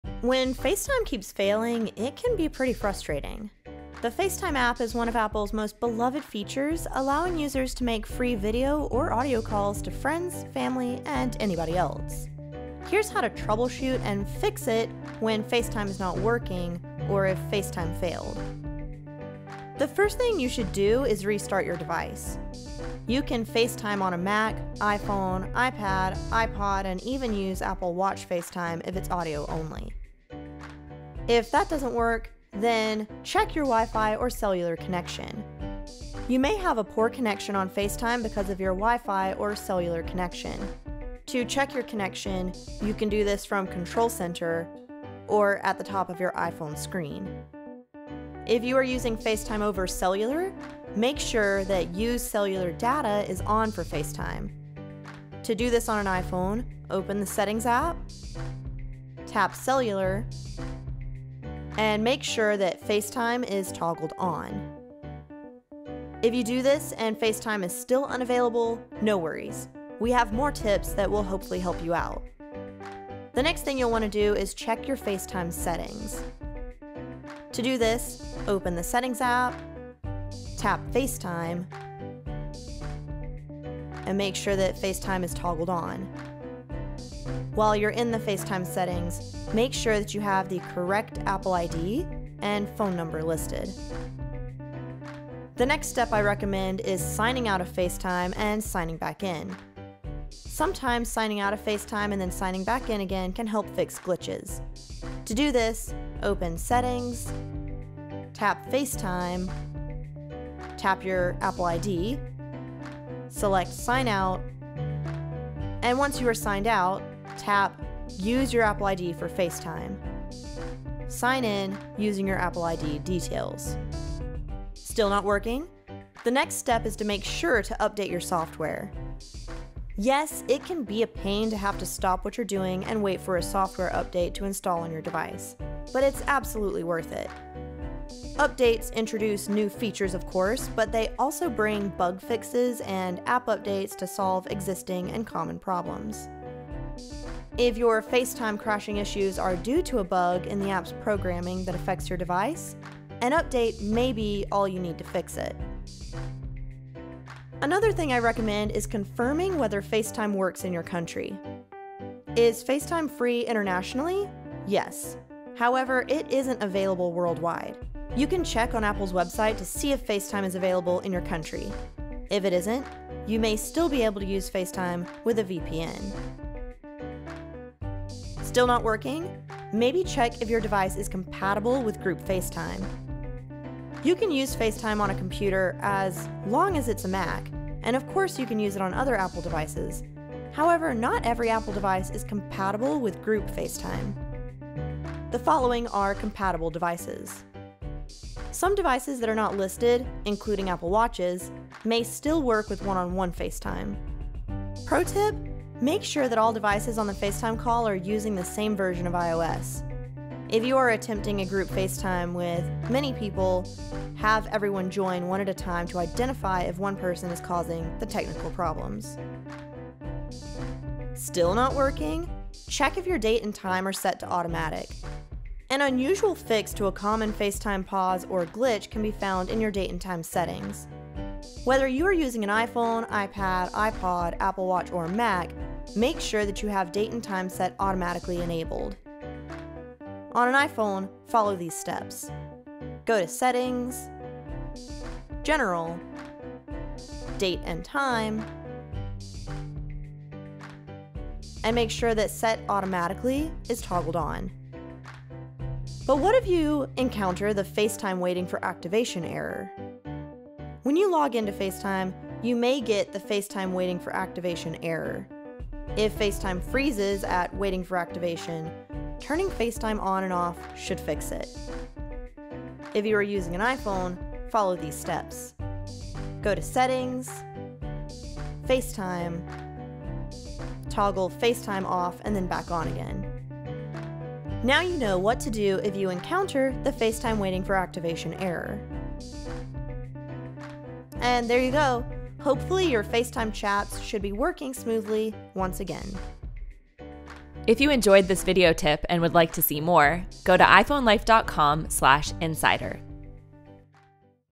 FaceTime Not Working? 10 Ways to Fix It in iOS 16
If FaceTime keeps failing on iPhone, iPad, or Mac, here's how to get it working again.

When FaceTime keeps failing, it can be frustrating. The FaceTime app is one of Apple’s most beloved features, allowing users to make free video or audio calls to friends, family, and anyone else using an iPhone, iPad, iPod, Apple Watch, or Mac. Here’s how to troubleshoot and fix it when FaceTime is not working or if FaceTime failed.

1. How to Restart iPhone, iPad, Mac & iPod If FaceTime Keeps Failing
FaceTime not working on iPhone, iPad, iPod, or Mac? The first thing you should do is restart your device. You can FaceTime on a Mac, iPhone, iPad, iPod, and even use Apple Watch FaceTime if it's audio-only.

How to restart each FaceTime-compatible device:

How to reset iPhone, iPod, or iPad
How to reset Apple Watch
To restart your Mac, go to the Apple Menu in the upper-left corner and select Restart.

2. Check Your Wi-Fi or Cellular Connection
You may have a poor connection on FaceTime because of your Wi-Fi or cellular connection. Check that the device you are using has a Wi-Fi or cellular connection; you can check this in the Control Center or at the top of your iPhone screen.

If you are using FaceTime over cellular, make sure that Use Cellular Data is on for FaceTime. To do this on an iPhone: